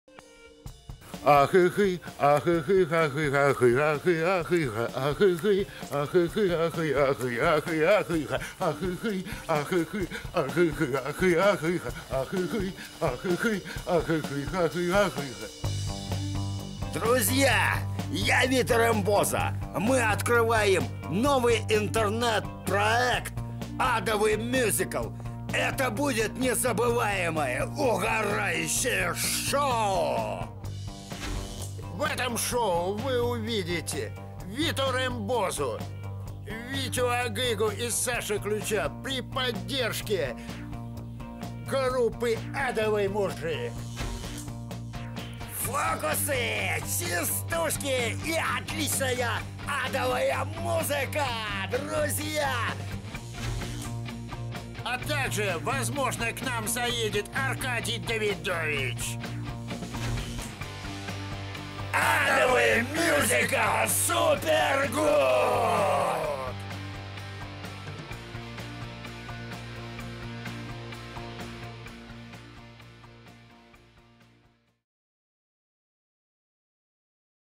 Новый проект "Адовый Мьюзикл" Hells Musical
В главных ролях: Миша Генератор драйва, Вито Рембозо, Саша Ключ,Аркадий Давидович.
Музыкальное сопровождение группа "Адовый Мужик".
О vip гостях данного мероприятия, будет сообщено дополнительно.